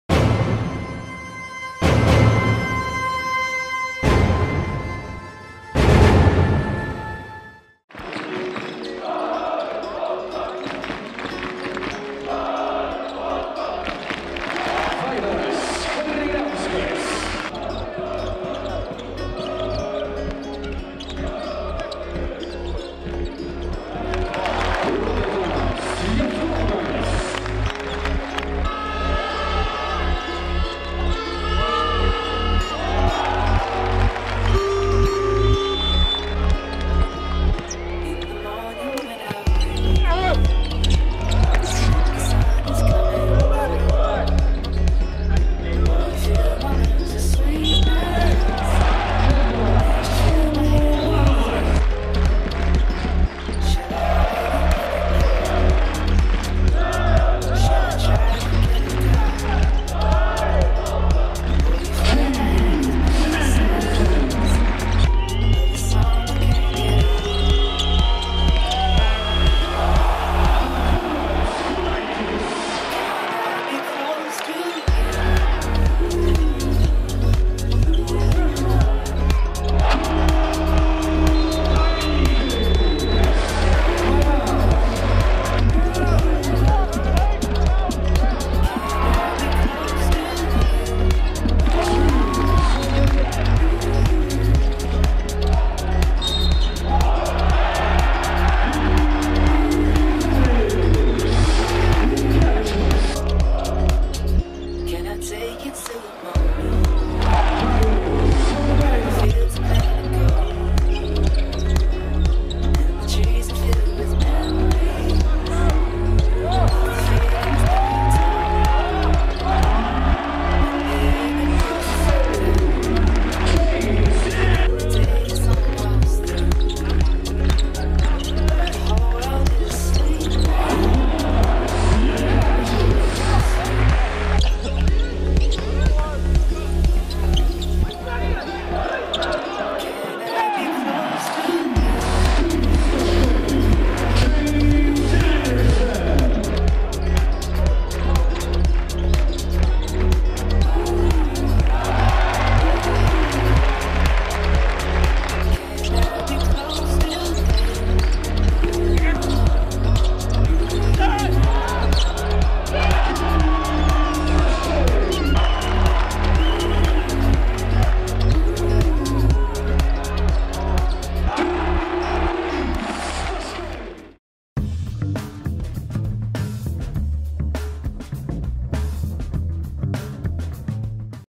Gražiausi rungtynių epizodai: Žalgiris - Lokomotiv
Kauno "Žalgirio" krepšininkai laimėjo septyniasdešimtmečio proga surengtą Kauno "Akropolio“ taurės turnyrą. Beveik idealią gynybą pademonstravę žalgiriečiai nugalėjo  Krasnodaro "Lokomotiv-Kuban“ komandą 65:49.